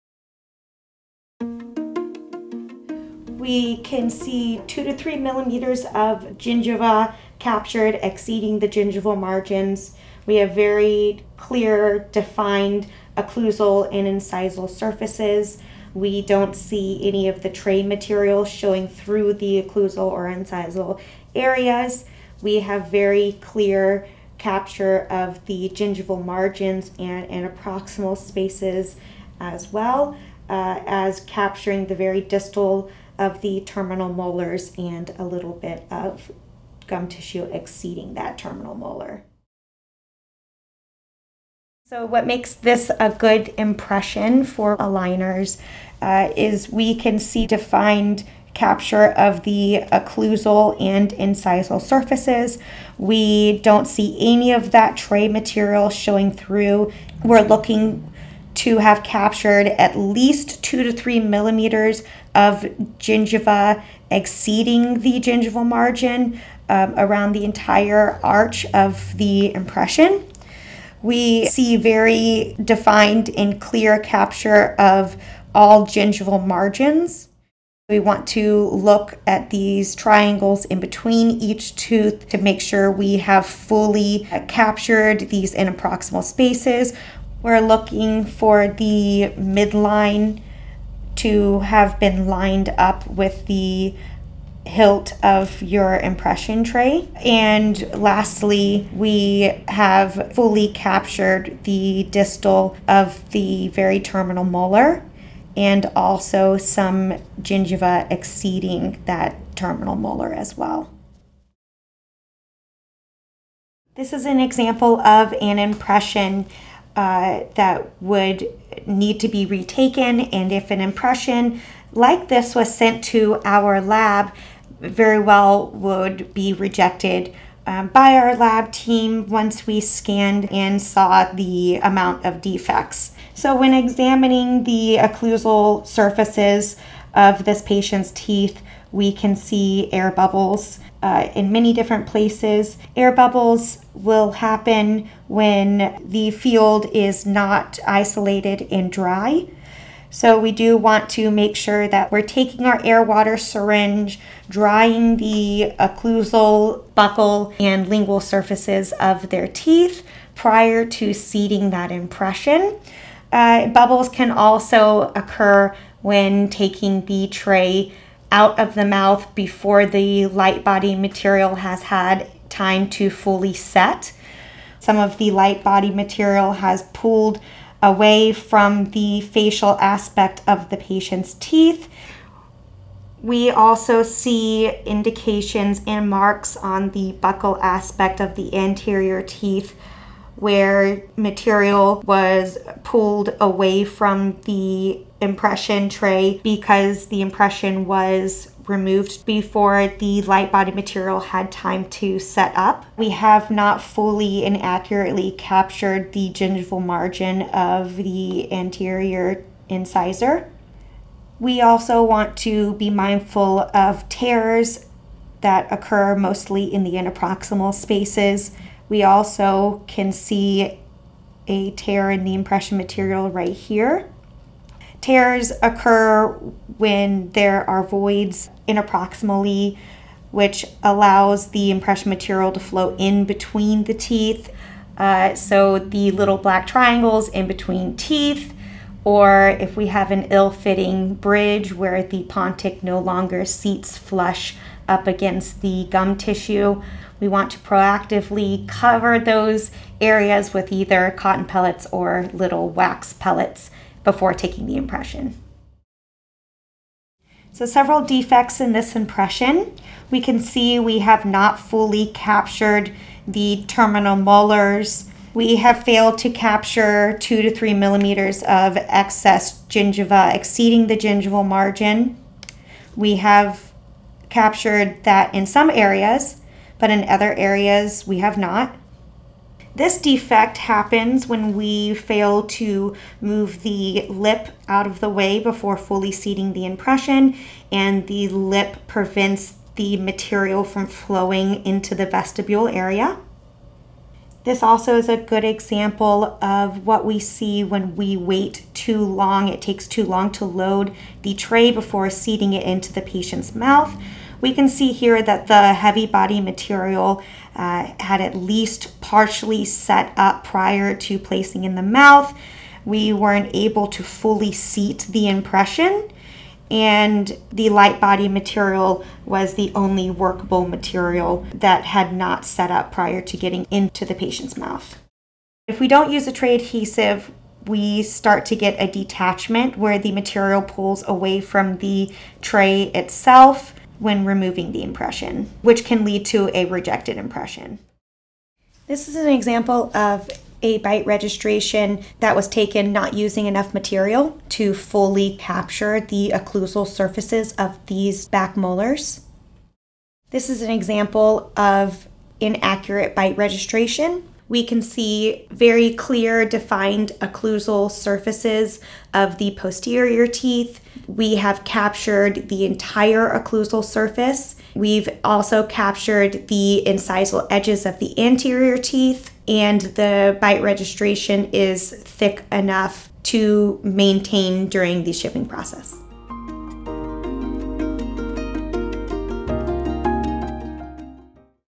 Assessing PVS Impressions for Reveal® Clear Aligners
Learn how to assess PVS impressions for Reveal Clear Aligner case submissions, and discover what makes an accurate impression for successful treatment planning versus an inaccurate impression that may be rejected by our lab.

00:00 Good Example # 1 - PVS Impression
00:39 Good Example # 2 - PVS Impression
01:51 Poor Example # 1 - PVS Impression
04:15 Poor Example # 2 - PVS Impression

05:39 Poor Bite Registration Example
05:49 Good Bite Registration Example